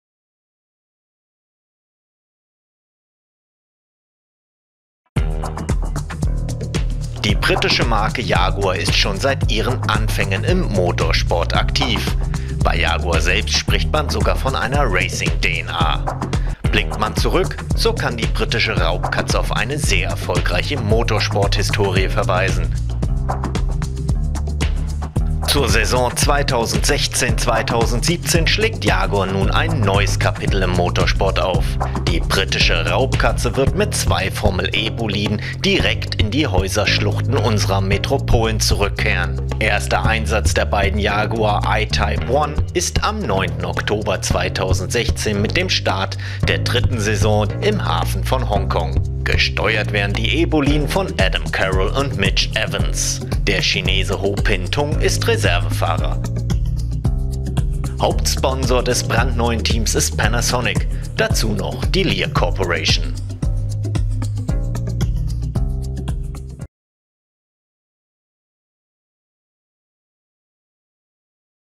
Jaguar I-Type
Die Marke Jaguar steht für Emotion und vor allem auch für puren Motorsport. Bis in die 1990iger Jahre war die Raubkatze ein überaus erfolgreiches Team in Le Mans. 2016 kehrt Jaguar mit dem I-Type in den Motorsport zurück. Die Briten starten mit eigenem Team in der Formel E. Startschuss für den brandneuen Jaguar I-Type ist am 9. Oktober in Hongkong.

Das Footage zum Beitrag stammt von Jaguar Deutschland.

Bei dieser Rubrik arbeitet das Team von mit zusammen.